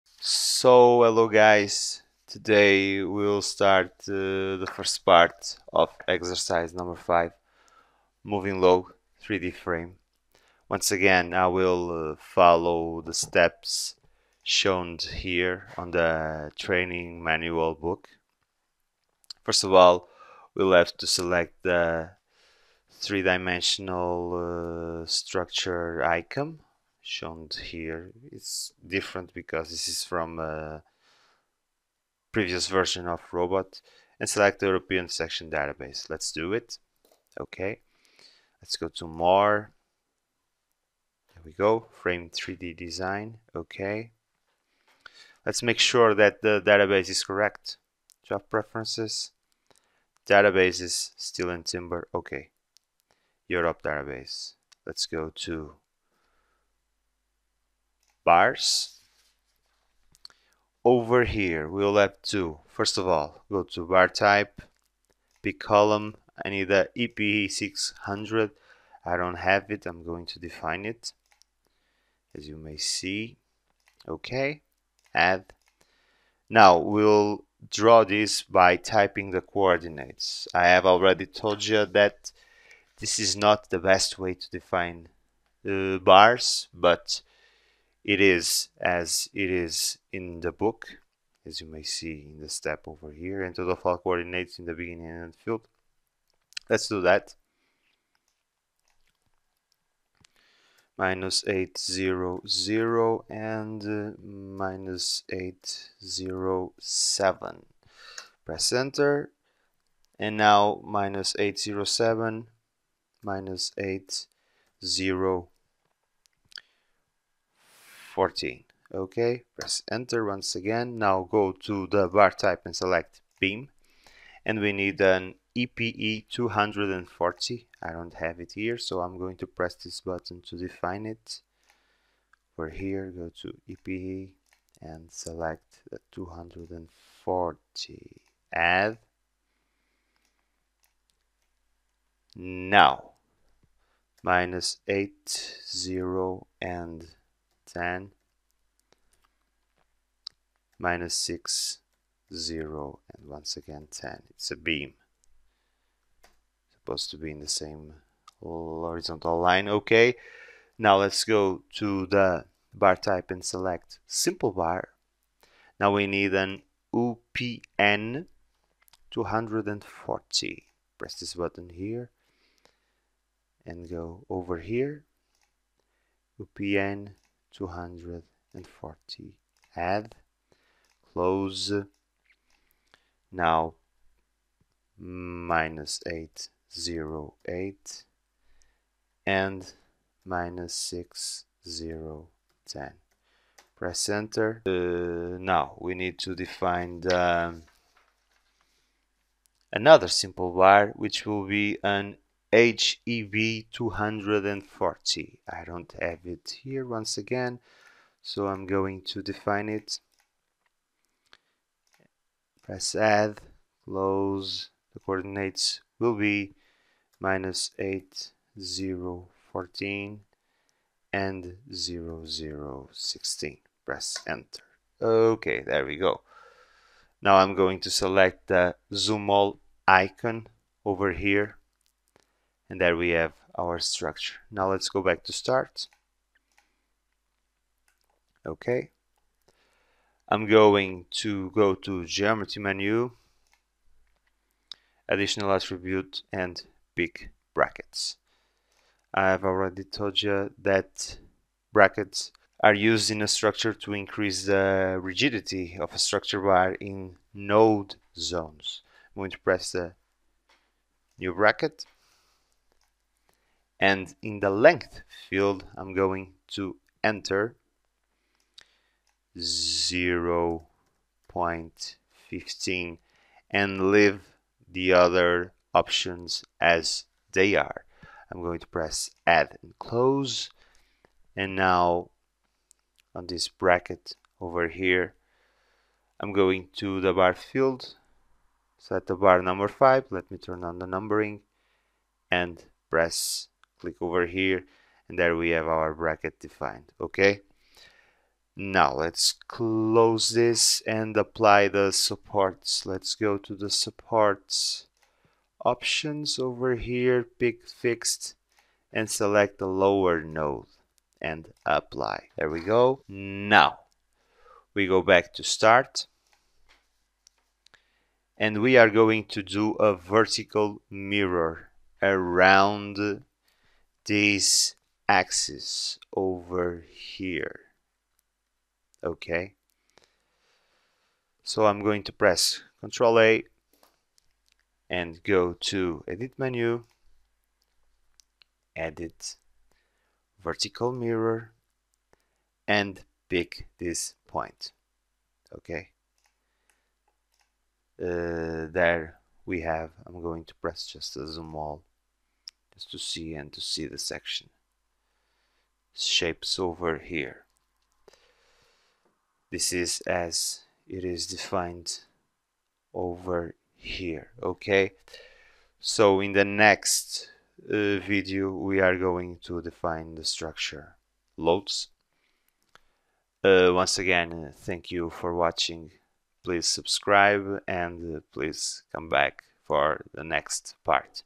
Exercise 5 - Part 1 - Moving Load - 3D Frame (Robot Structural Analysis)
First part of the exercise on moving loads on a 3D frame, with Robot Structural Analysis Professional.